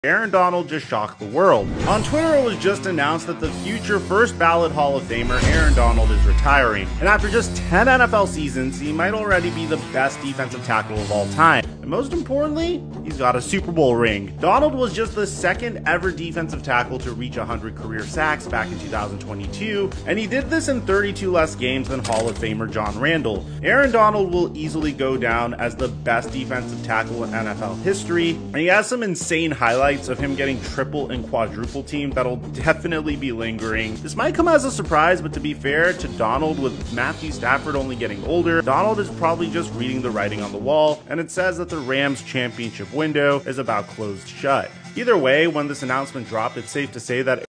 AARON DONALD RETIRED 🚨‼️🏆
Get ready to say goodbye to one of the greatest defensive tackles in NFL history! Aaron Donald has shocked the world by announcing his retirement after just 10 incredible seasons in the league. With 10 Pro Bowl selections, 8 first-team All-Pro selections, 3 Defensive Player of the Year awards, and a Super Bowl ring, Donald's legacy is unmatched. His dominance on the field, often facing triple and quadruple teams, will be remembered for years to come. While his retirement may come as a surprise, Donald may be sensing the end of the Rams' championship window.